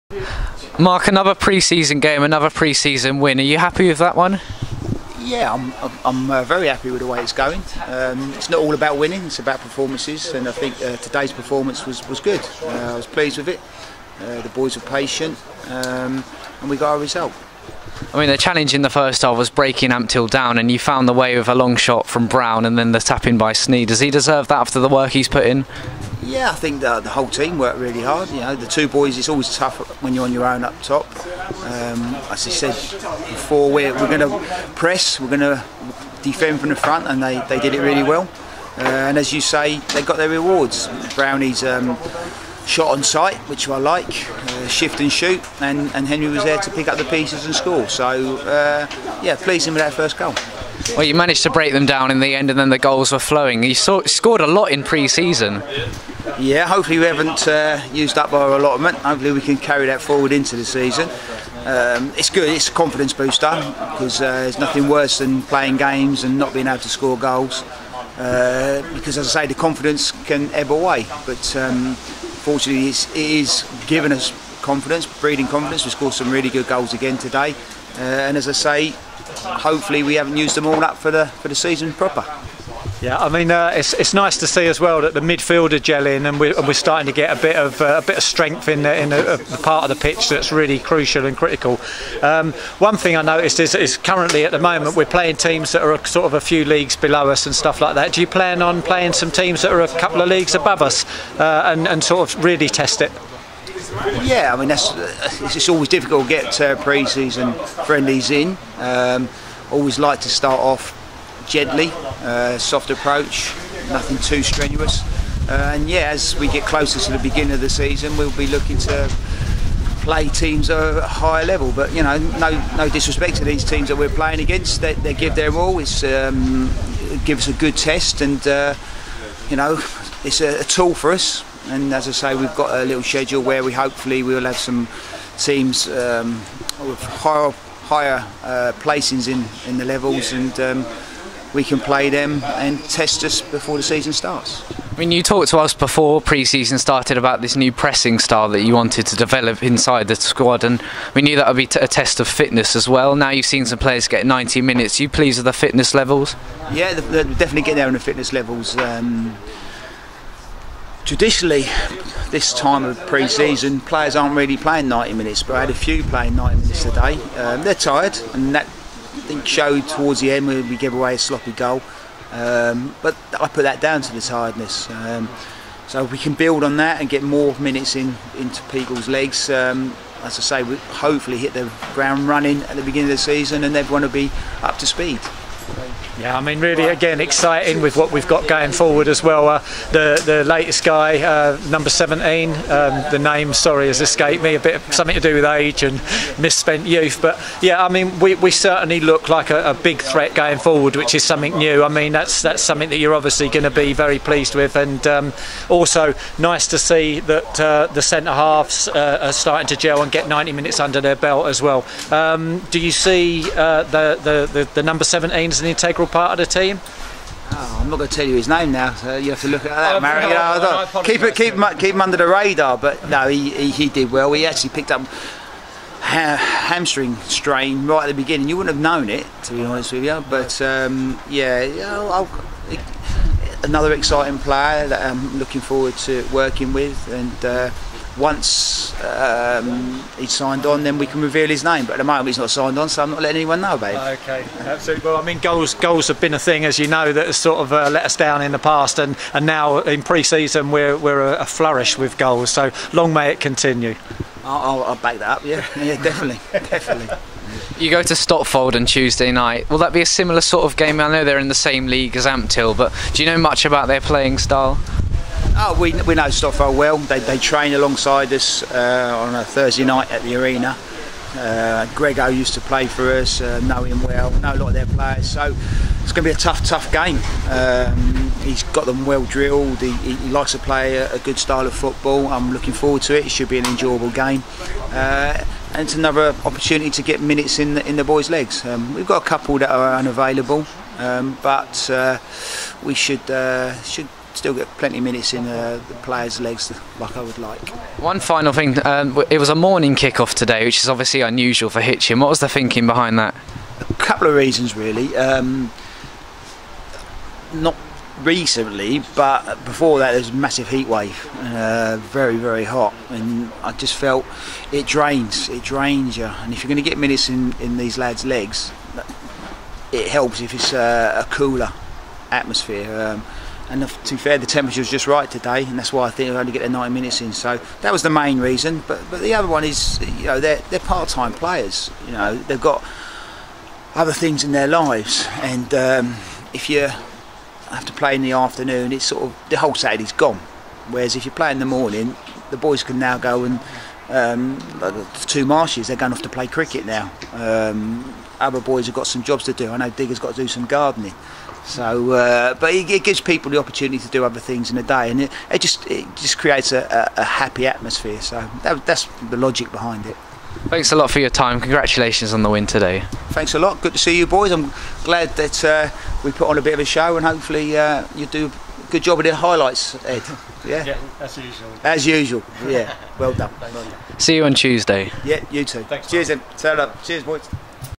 Mark Burke Post Match Interview vs Ampthill Town
Here are the thoughts of manger Mark Burke after the 5-1 win over Ampthill Town.